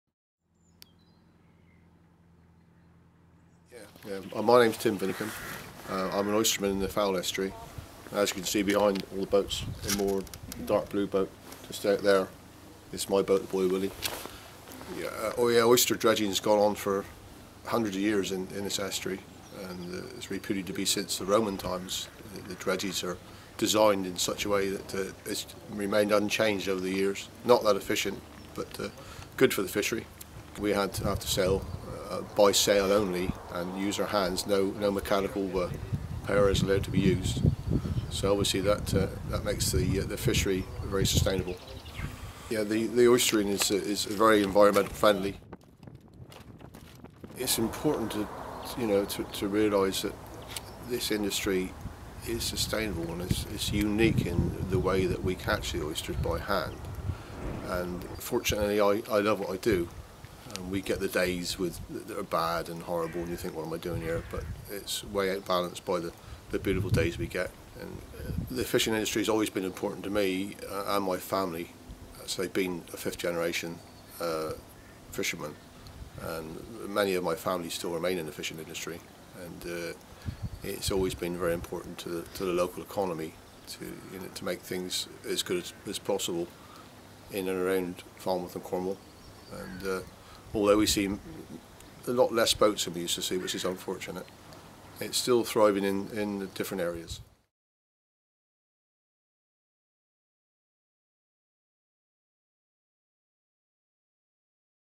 Meet the fisherman, Tim Vinnicombe
Tim is an oyster dredger working out of Mylor. He works under sail, by hand using no machinery. He uses the same techniques that have been passed down through 5 generations of his family. Fal oysers are recommended by Cornwall Good Seafood Guide Find out more y by visiting